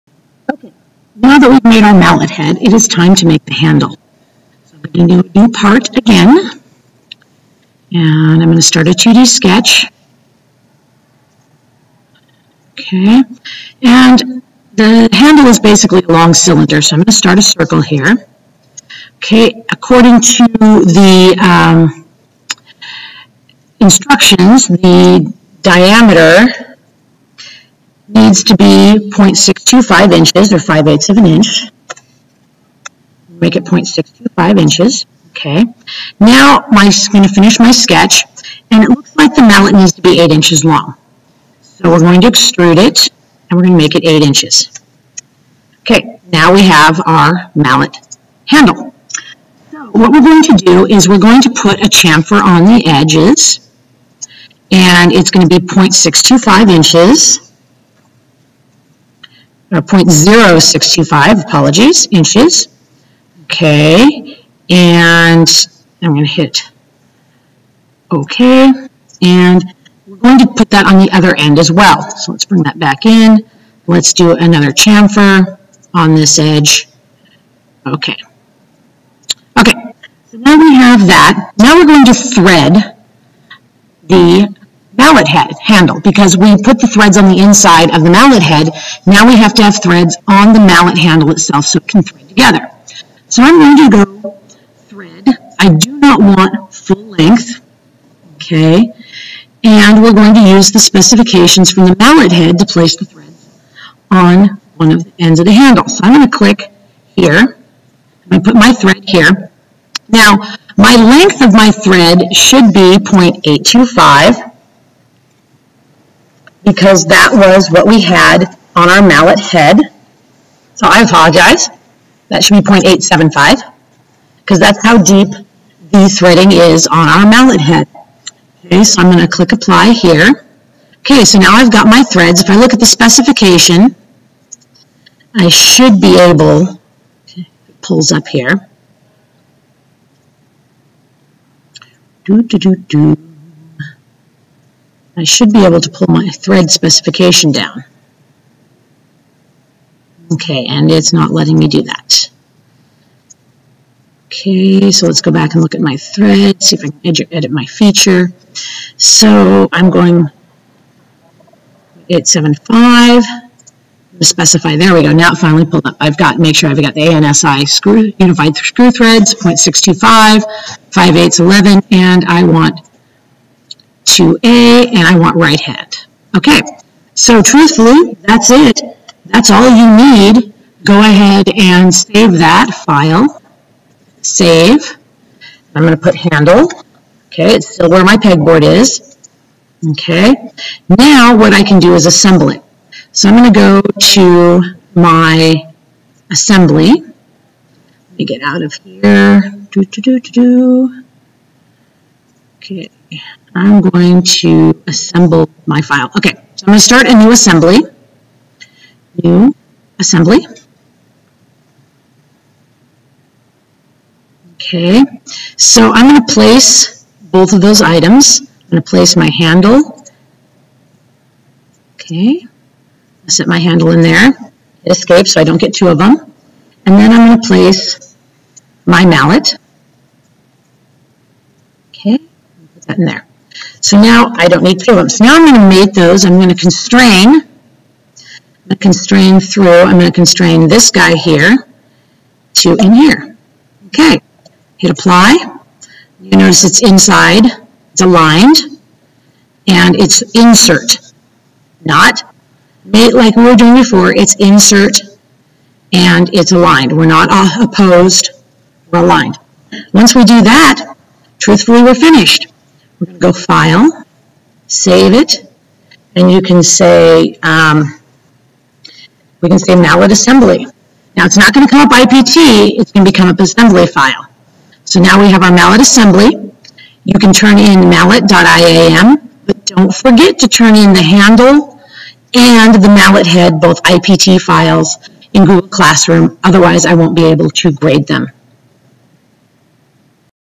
Mallet Handle Assembly
How to create the handle and assemble the mallet